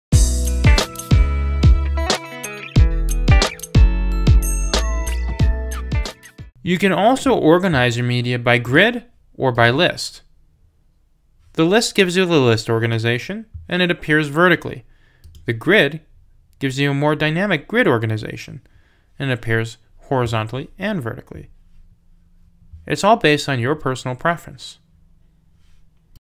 How to Organize Your Media Library
Learn how to organize your media library in WordPress. Ordering and organizing your media library will help keep your files in order and help prevent 404 error pages.